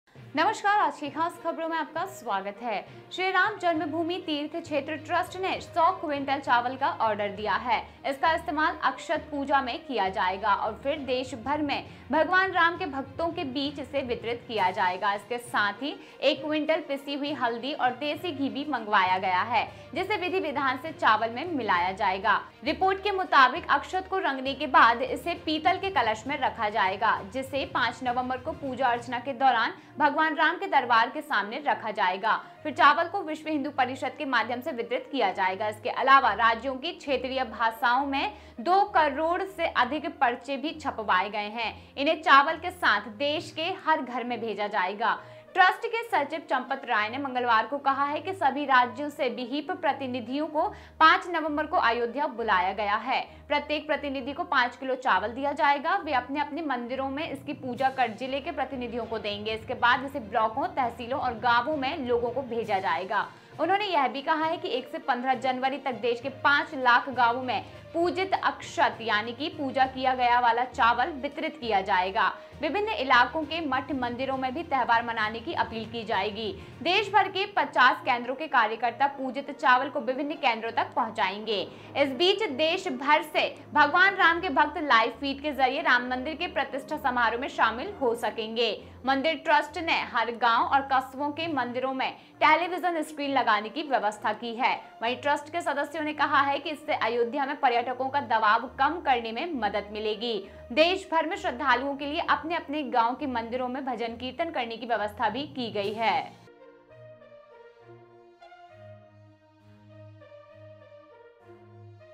Ram Mandir में 5 November को पूजे जाएंगे 100 Quintal चावल, Shri Ram Janmabhoomi Trust ने दिया Order!
श्री राम जन्मभूमि तीर्थ क्षेत्र ट्रस्ट ने 100 क्विंटल चावल का ऑर्डर दिया है. इसका इस्तेमाल "अक्षत पूजा" में किया जाएगा और फिर देश भर में भगवान राम के भक्तों के बीच इसे वितरित किया जाएगा. इसके साथ ही एक क्विंटल पिसी हुई हल्दी और देसी घी भी मंगवाया गया है, जिसे विधि-विधान से चावल में मिलाया जाएगा.

Shri Ram Janmabhoomi Teerth Kshetra Trust has ordered 100 quintals of rice. It will be used in "Akshat Puja" and then distributed among the devotees of Lord Ram across the country. Along with this, one quintal of ground turmeric and desi ghee has also been ordered, which will be mixed with rice as per the procedure.